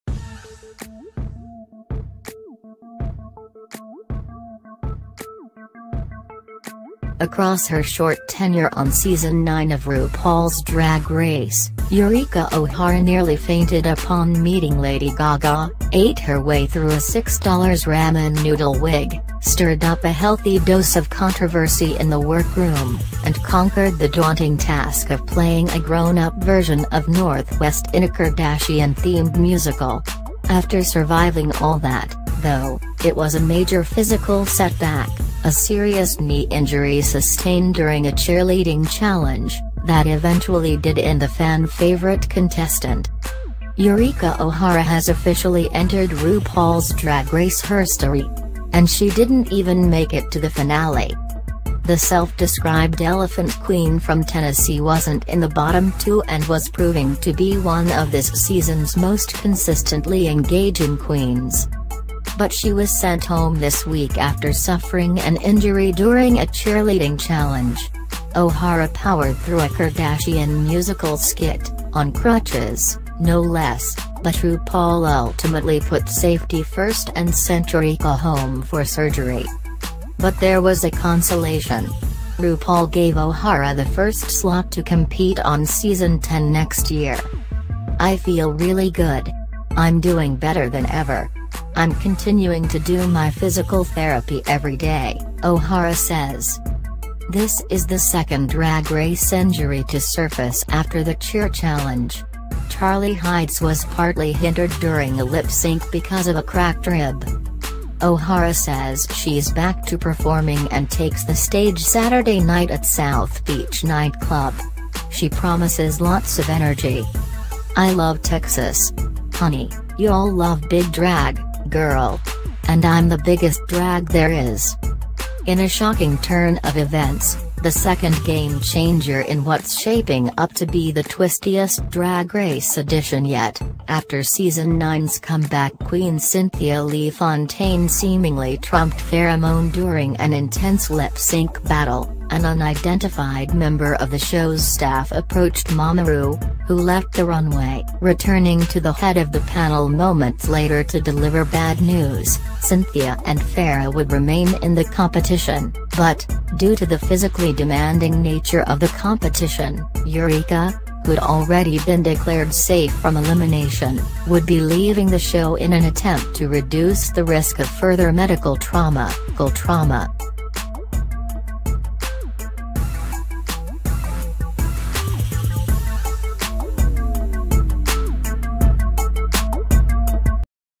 'Drag Race' star Eureka talks shock elimination, returning for Season 10
Across her short tenure on season 9 of RuPaul’s Drag Race, Eureka O’Hara nearly fainted upon meeting Lady Gaga, ate her way through a $6 Ramen noodle wig, stirred up a healthy dose of controversy in the work room, and conquered the daunting task of playing a grown-up version of North West in a Kardashian-themed musical. After surviving all that, though, it was a major physical setback — a serious knee injury sustained during a cheerleading challenge — that eventually did in the fan favorite contestant.
She was sent home this week after suffering an injury during a cheerleading challenge.